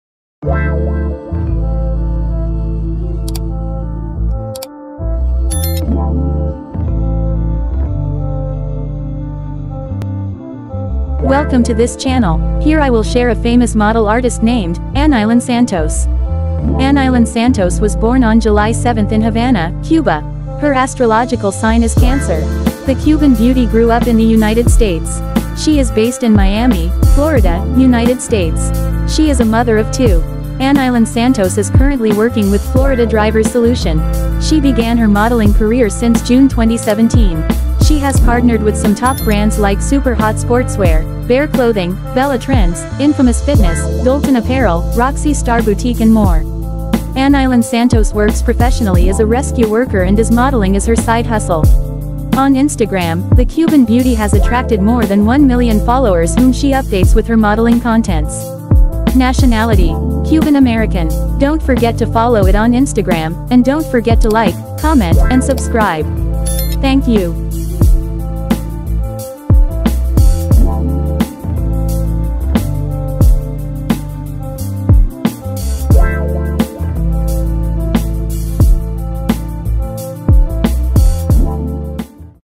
Anailen Santos 🇺🇸...| Cuban-American Curvy Plus-sized Model | Beautiful Fashion Model | Biography
Original content owner: @Anailen Santos

Anailen Santos is a Cuban-American Rescue Worker, Entrepreneur, Fashion Model, Social Media Personality, Digital Creator, Influencer, Avid Traveler and Instagram Celebrity who is well known for rescuing car accident victims. Anailen Santos was born in Havana, Cuba. Her astrological sign is Cancer.